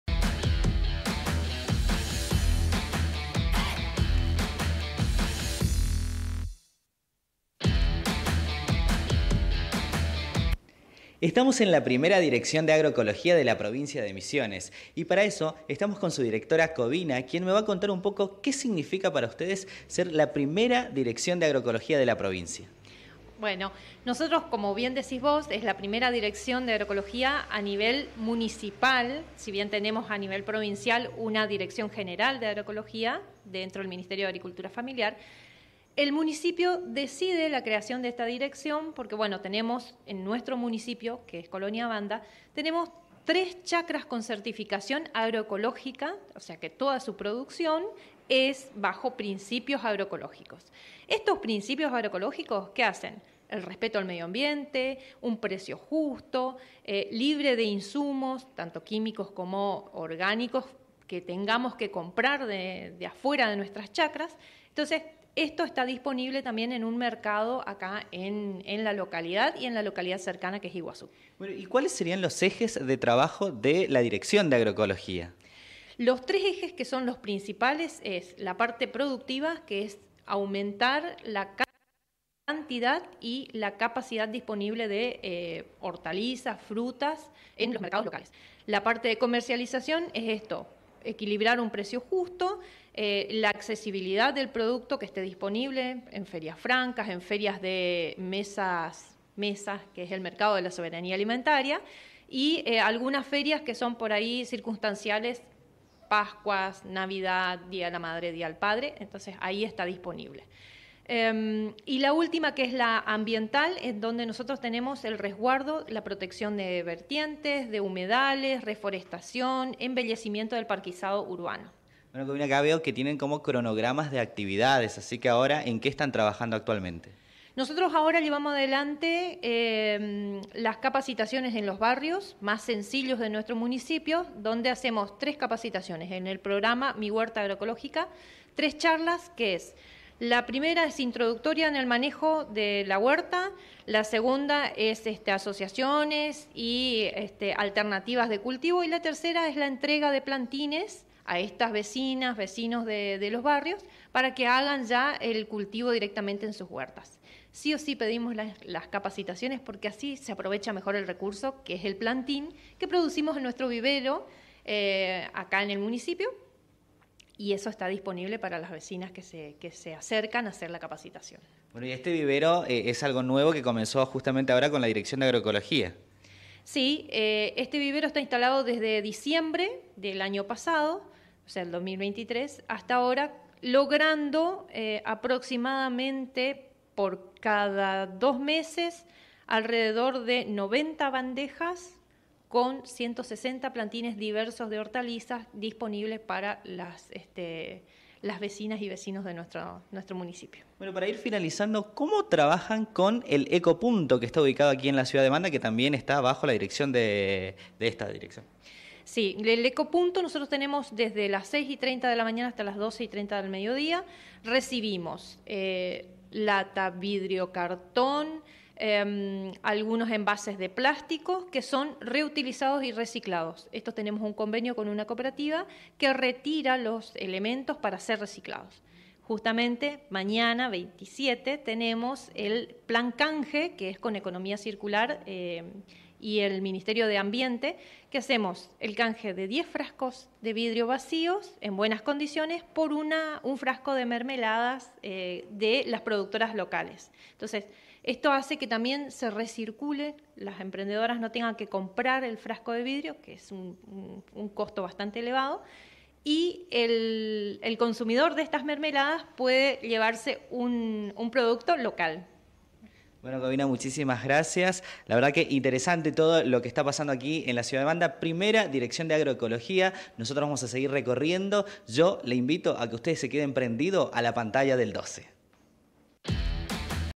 TuCiudadNuestraCasa: Wanda fortalece a la producción local
Asesoran a productores de la zona.